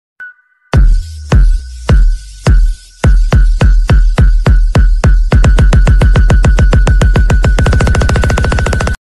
9 | JENEL LAUSA BOXING SPEED CHALLENGE ON TIKTOK
A few weeks ago, I received an invitation from the TikTok Sports & Fitness Philippines to be a part of their group and join their creator program.

Instagram & TikTok: @labanlausa

Halong kamo. Aton ini. 👊🏻